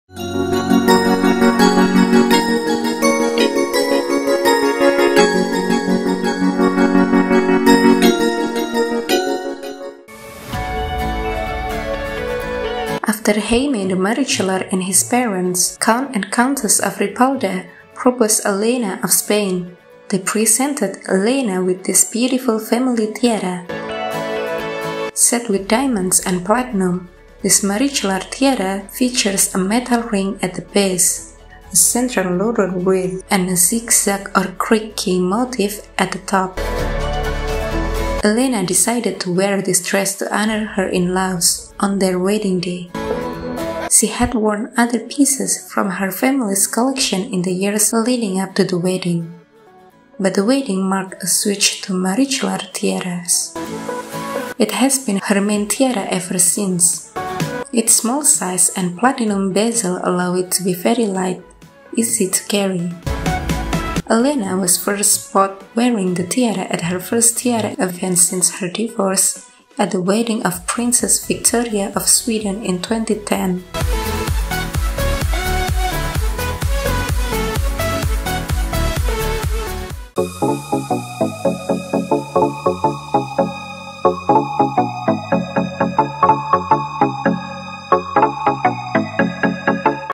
Spain Royal Tiara | The Marichalar Meander Tiara
After Jaime de Marichalar and his parents, Count and Countess of Ripalda, proposed to Elena of Spain, they presented Elena with this beautiful family tiara. Set with diamonds and platinum, this Marichalar tiara features a metal ring at the base, a central laurel wreath, and a zigzag or Greek key motif at the top.

Elena decided to wear this dress to honor her in-laws on their wedding day. She had worn other pieces from her family's collection in the years leading up to the wedding, but the wedding marked a switch to Marichalar tiaras. It has been her main tiara ever since. Its small size and platinum bezel allow it to be very light , easy to carry. Elena was first spotted wearing the tiara at her first tiara event since her divorce at the wedding of Princess Victoria of Sweden in 2010.